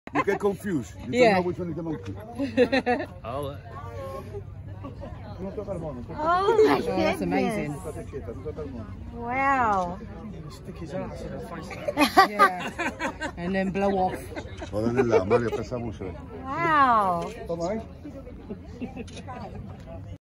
Gibraltar. Macaque, Monkey
As long as you have a treat they'll come by for a pic.  Our tour guide had peanuts in his pocket.